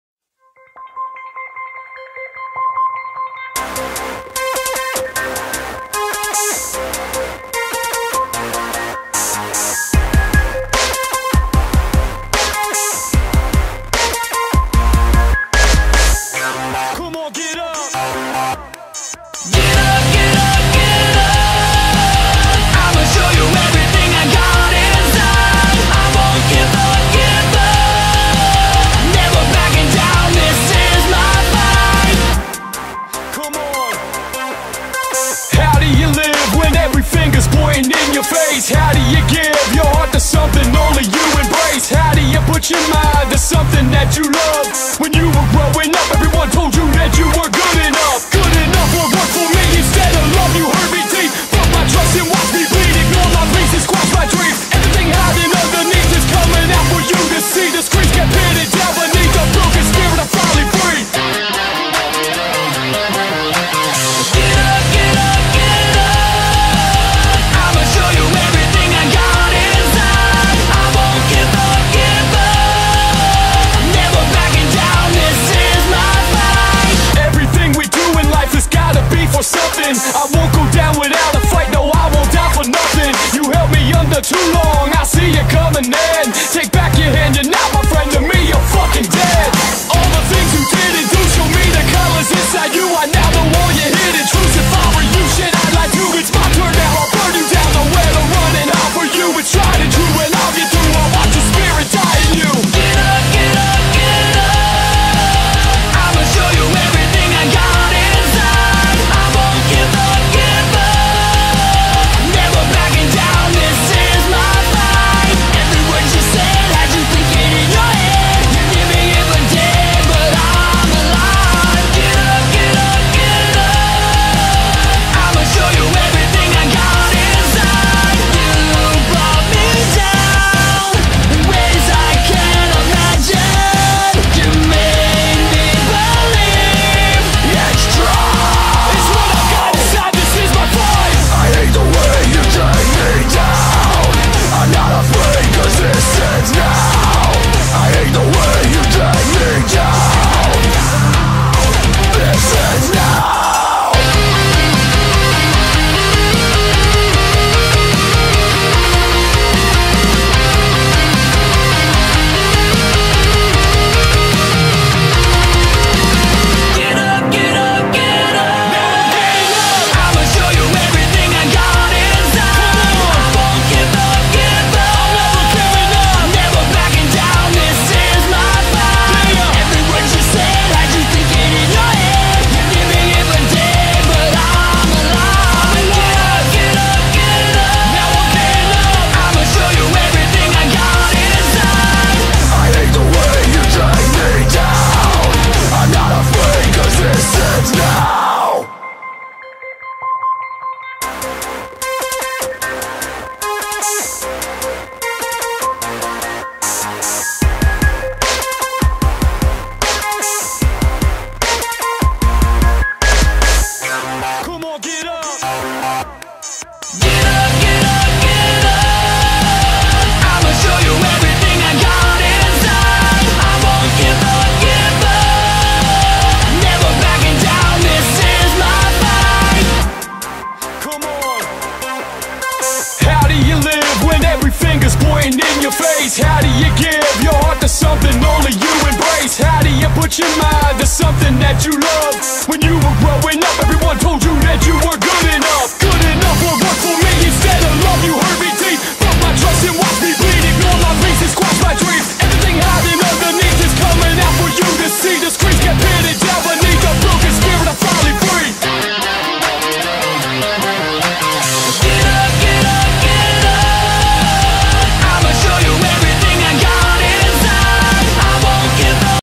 The Making Of Soundgarden Logo T Shirt Design | Silk Screen Printing and Handmade Painting
Silkscreen printing from paper cut out and handmade painting.

Artist: From Ashes To New